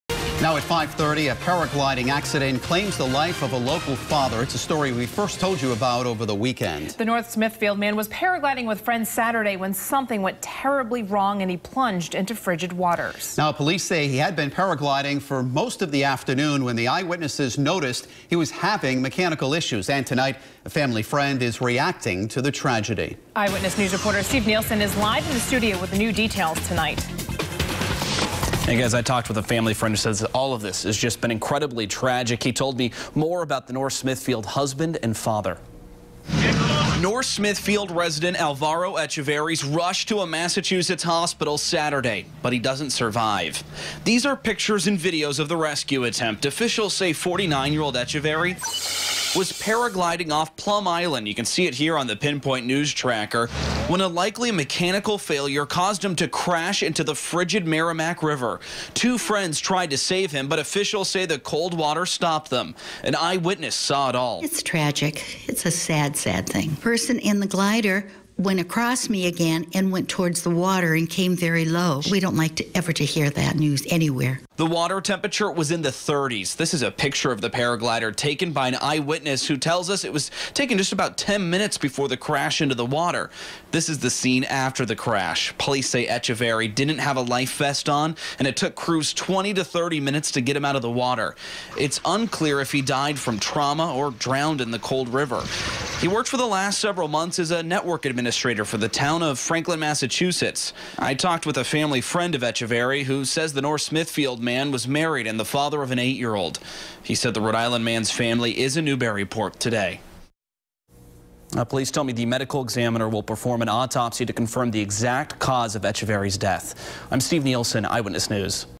Police ID RI man killed in paragliding accident
Officials have identified a Rhode Island man killed in a weekend paragliding accident. It happened in the waters off Plum Island in Newburyport, Mass.